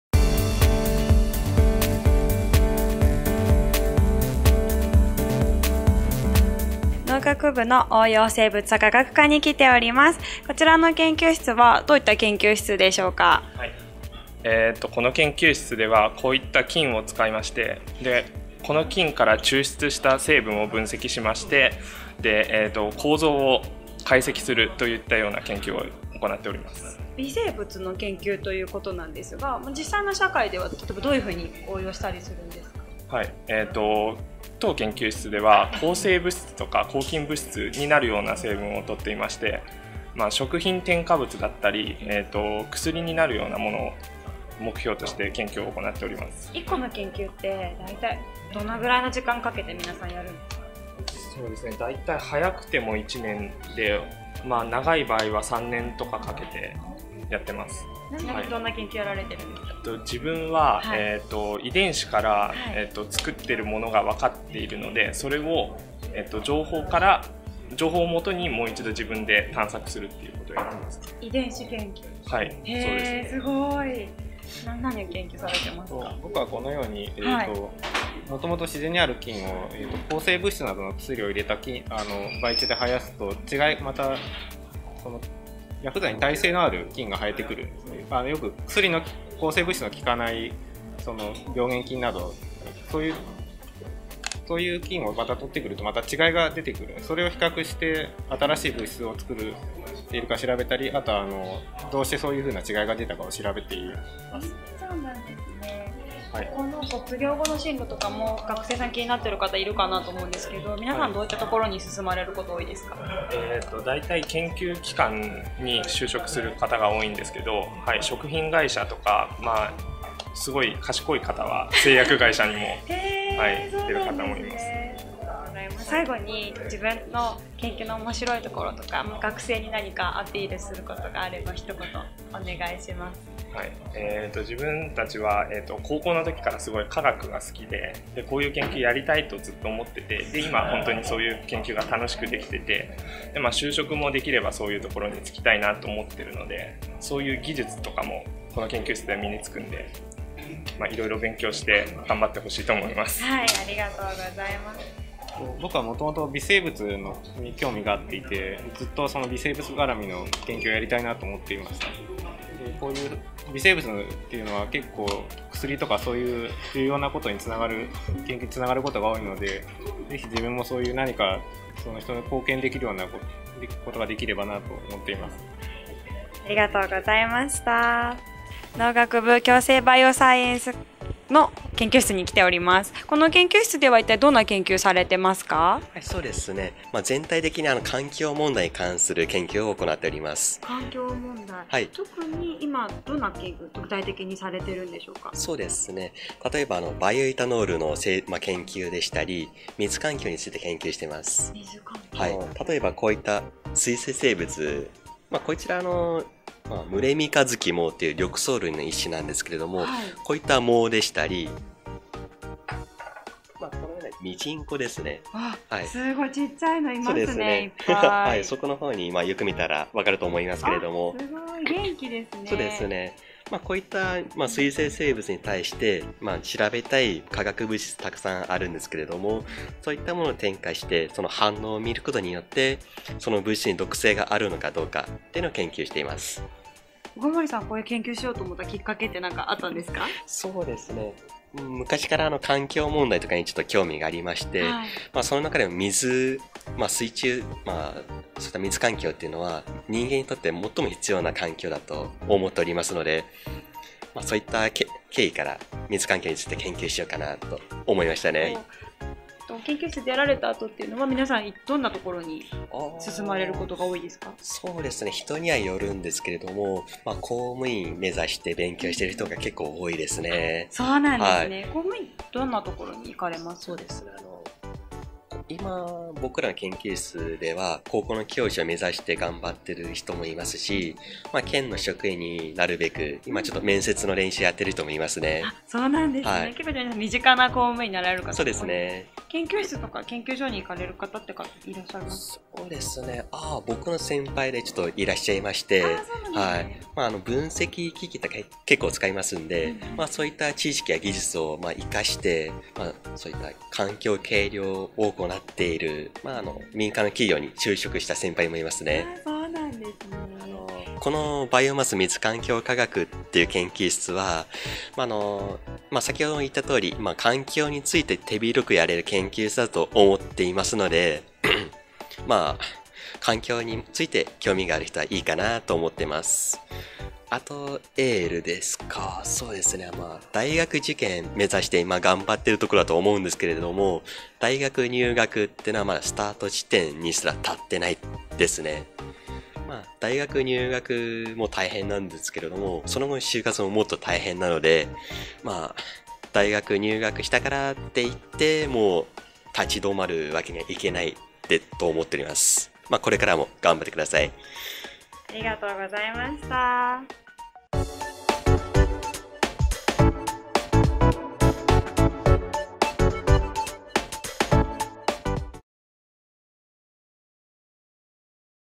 農学部生達の研究活動を紹介! 応用生物化学科 共生バイオサイエンス学科 農学部 夏季オープンキャンパス 2014 - 静岡大学
平成27年度の応募要項は下記サイトをご覧ください！